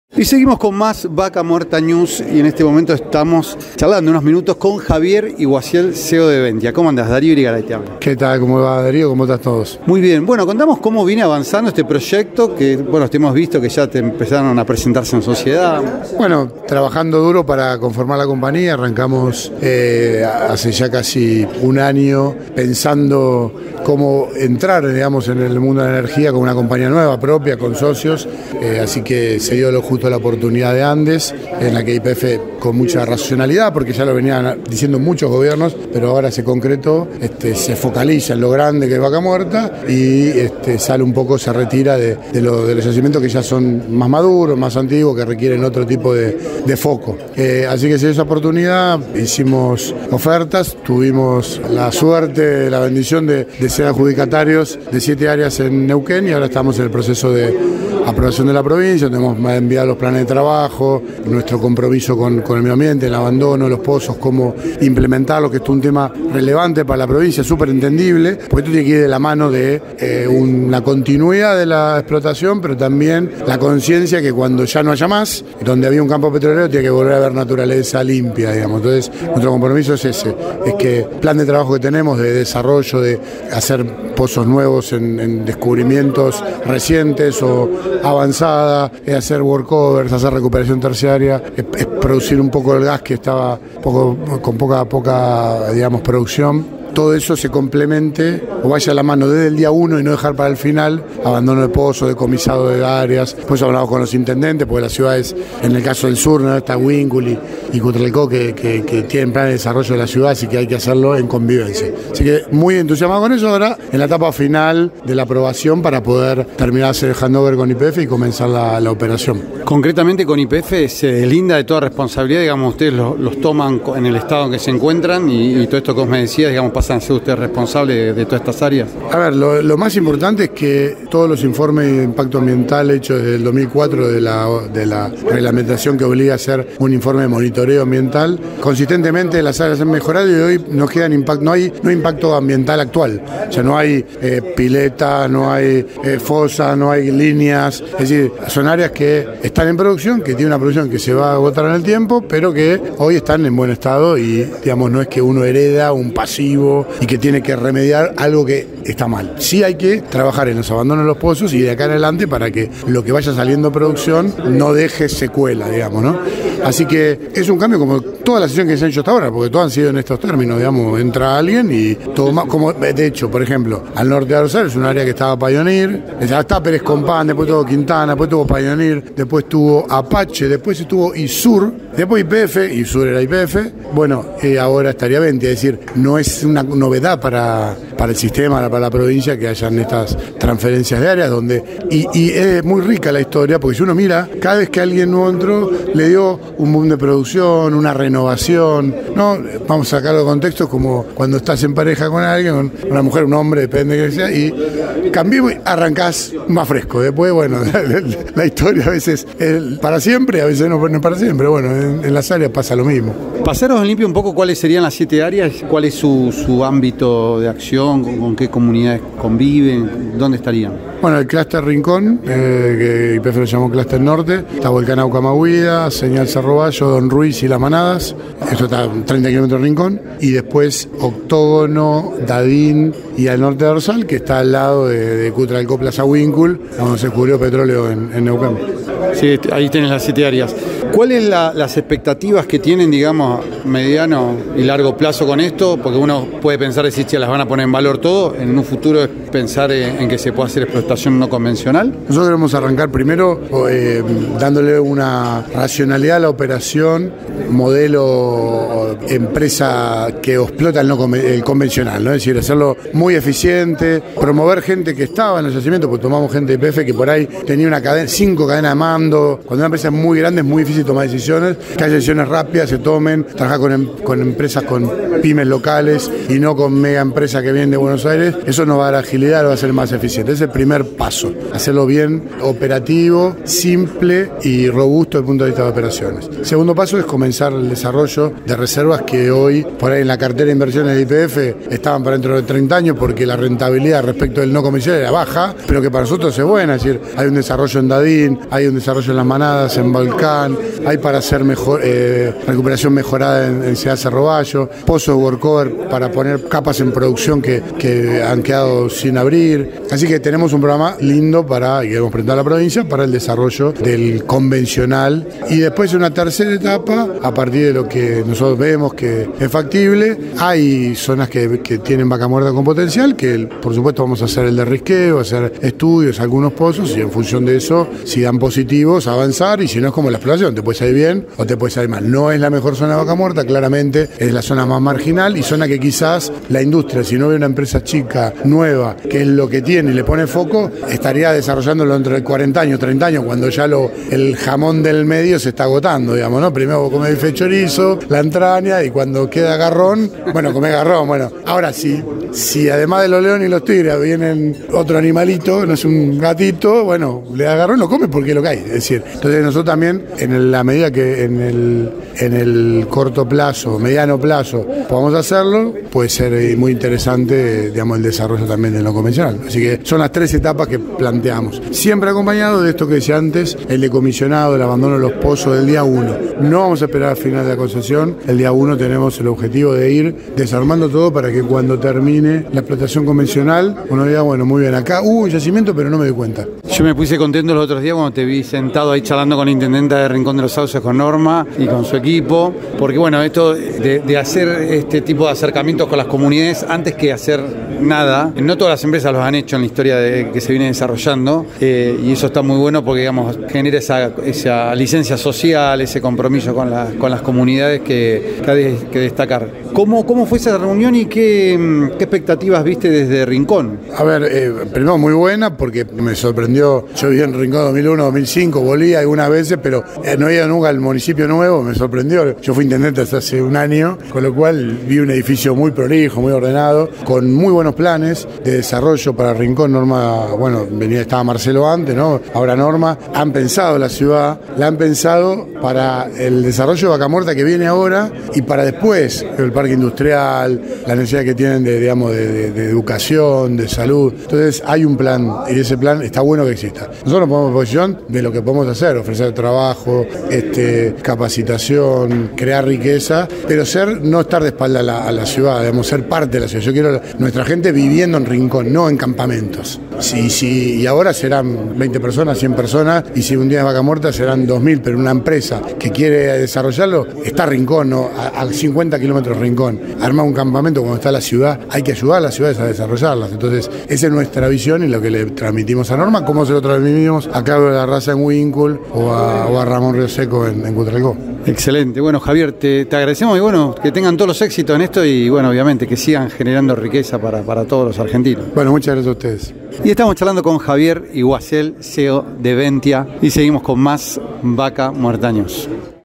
Bentia Energy: Nuevos Desafíos y Compromiso Ambiental en Vaca Muerta
Javier Iguacel (CEO Bentia Energy) Vaca Muerta News